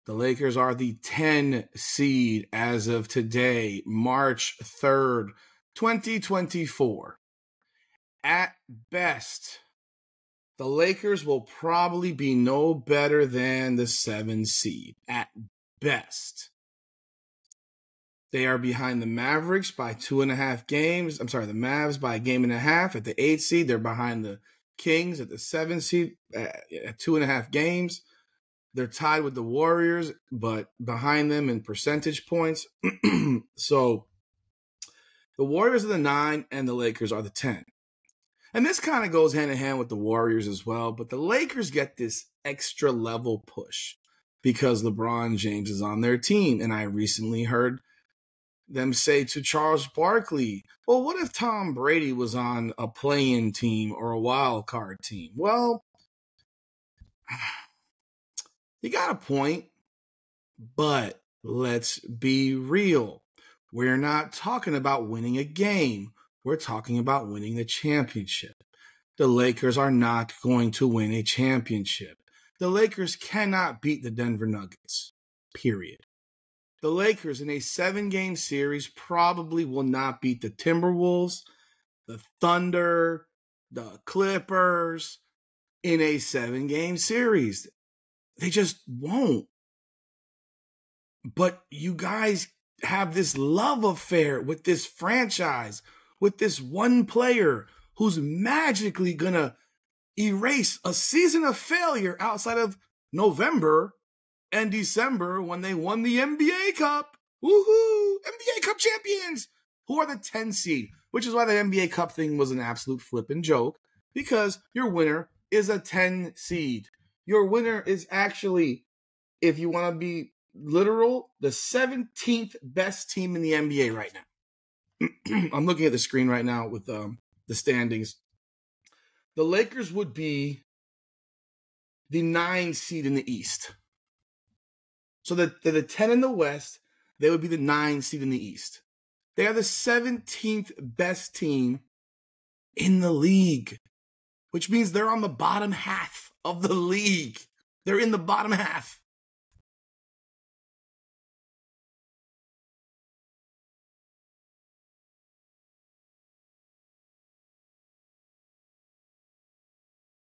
The Los Angeles Lakers are Garbage! Accept it!
The Los Angeles Lakers are not good. Accept it. They are are first round playoff team at beast and then will be on the couch watching with the rest of us. The Los Angeles Lakers are the 10 seed right now. The media keeps pushing a ridiculous narrative. Only because of Lebron James does this narrative continue to get pushed. Stop the noise! The Lakers are done! Comment and let us know your thoughts!